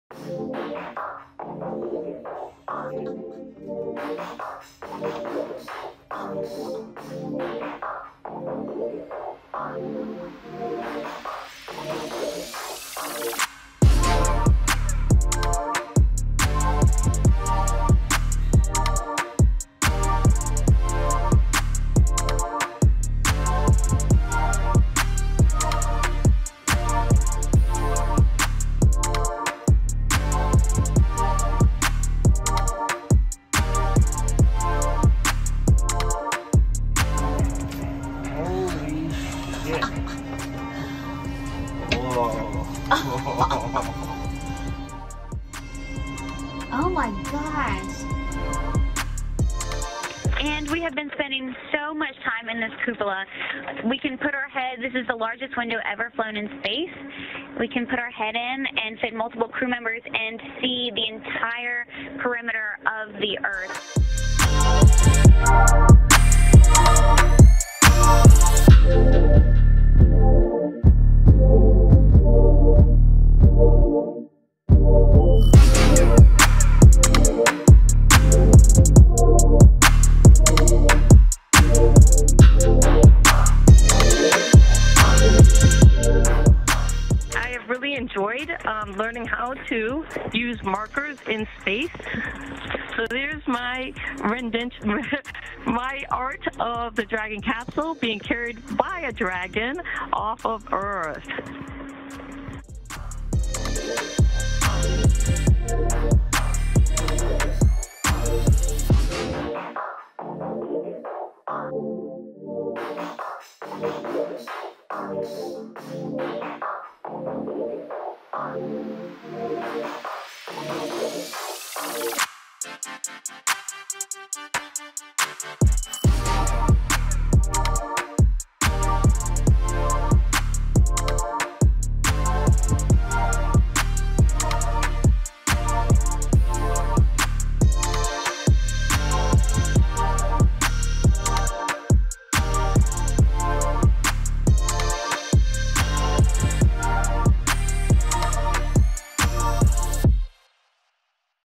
SpaceX : revivez le moment où les quatre touristes ont découvert la Terre depuis l’espace
Aucune coupole vitrée de cette taille n’a orbité autour de la planète bleue avant cette mission. « On passe énormément de temps ici », glousse Hayley Arcenaux, éblouie par la vue qui s’offre à elle dès qu’elle lève les yeux vers l’immense fenêtre. Ils sont quatre Américains. Quatre touristes à avoir vécu une expérience unique . La première en son genre : emmener quatre personnes lambdas dans le cosmos pendant trois jours, sans l’accompagnement d’un astronaute professionnel. Six mois d’entraînement plus tard, le défi est relevé. L’espace ne devrait plus être accessible qu’aux équipages longuement entraînés. Pour SpaceX, le tourisme spatial est l’avenir. Jouer de l’ukulélé, regarder des films, dessiner… Tant d’activités de voyage auxquelles Jared, Chris, Sian et Hayley ont pu participer, parfois réapprendre. Mais la mission « Inspiration4 » a surtout été l’occasion de mettre le corps humain à l’épreuve de l’apesanteur, de collecter des données autour du sommeil, de la pression artérielle, du rythme cardiaque. L’entreprise aérospatiale y voit le début d’une nouvelle ère. En janvier 2022, une navette SpaceX repartira vers l’espace avec trois nouveaux touristes à bord. Une mission d’une dizaine de jours sous le nom de « Ax-1 »